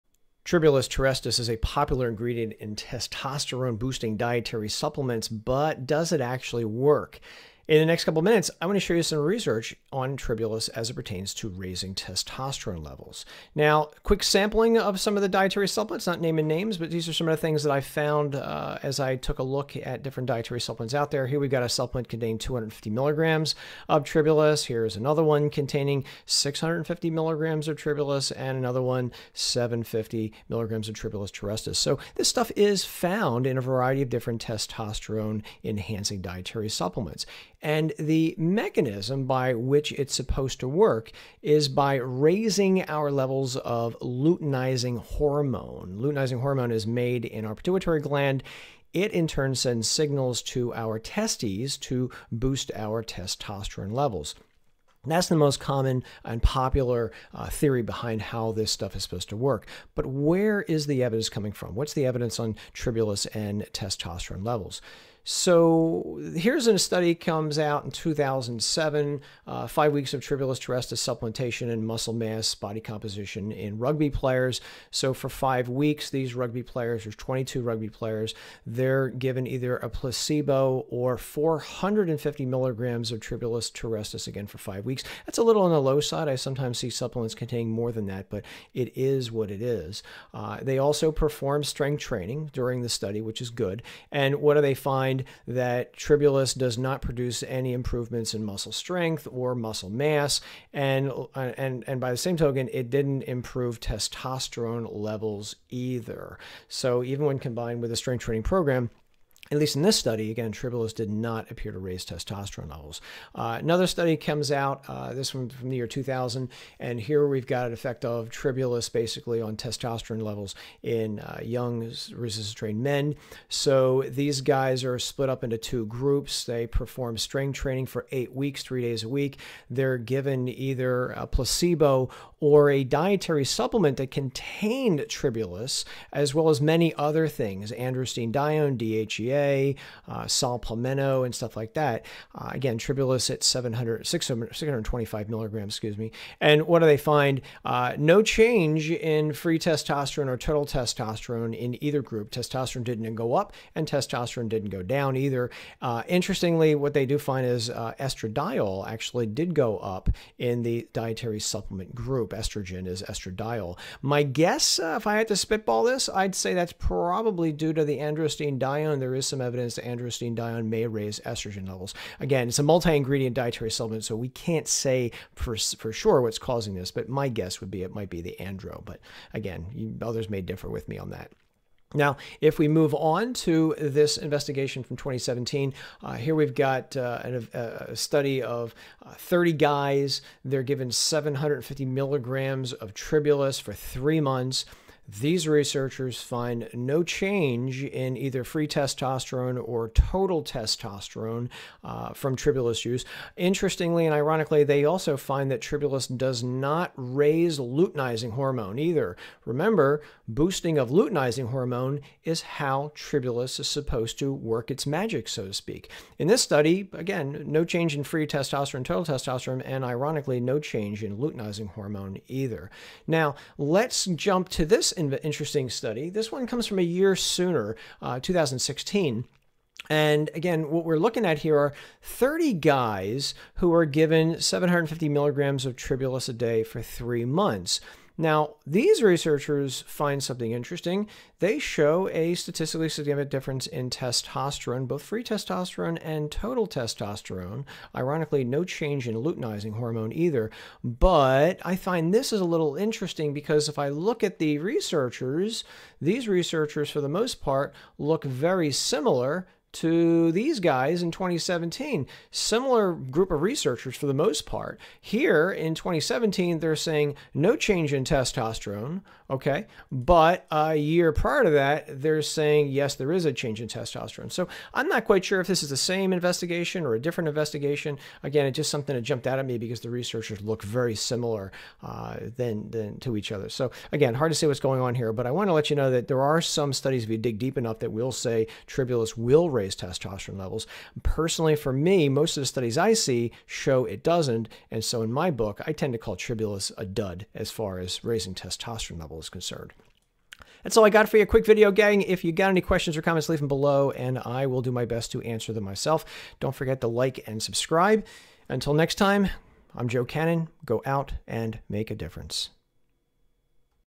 Can Tribulus Terrestris Raise Testosterone Levels?
Can Tribulus Terrestris boost testosterone levels or make you stronger? See the Tribulus research on how effective this reputed natural enhancer of anabolic hormones it really is. Decide for yourself if Tribulus is right for you. Just the facts.

🔴 🔴Things I like & use:
🔴🔴 🔴Things used to make this video:
1 Lot’s of work
2 This video editing software
3 This Microphone